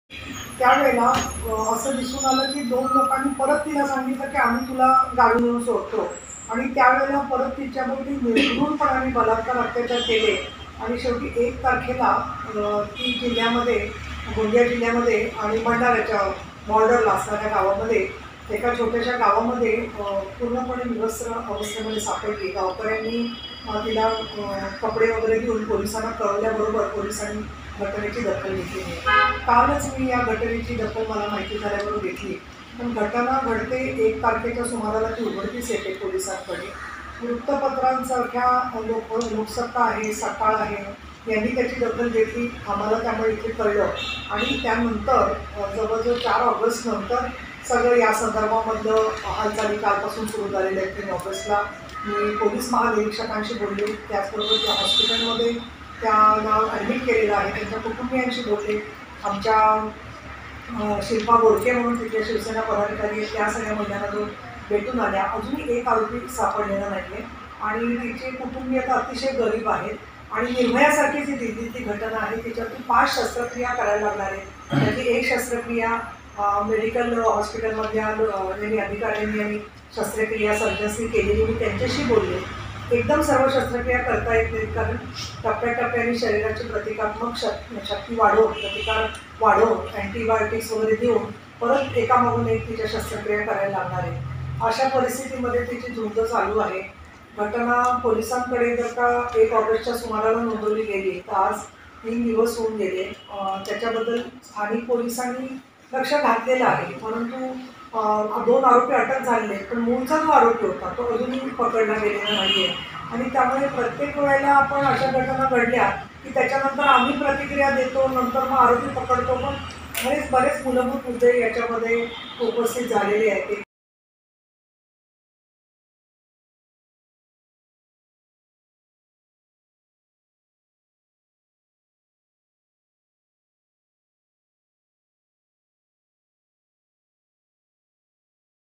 Bhandara l भंडारा प्रकरणावर नीलम गोऱ्हे म्हणाल्या... l LetsUpp Marathi
भंडाऱ्यात मदतीचे आश्वसान देऊन एका ३५ वर्षीय विवाहित महिलेवर सामुहिक अत्याचार केल्याची धक्कादायक घटना घडली. या पीडितेची प्रकृती गंभीर आहे. 'या पीडितेच्या ५ शस्त्रक्रिया कराव्या लागणार आहे. शिवाय ही घटना निर्भयासारखी असून पीडितेची झुंज सुरू असल्याचं त्या म्हणाल्या. मुख्यमंत्री उपमुख्यमंत्री यांनी या घटनेची दखल घेतली नाही.' अशी प्रतिक्रिया नीलम गोऱ्हे यांनी दिली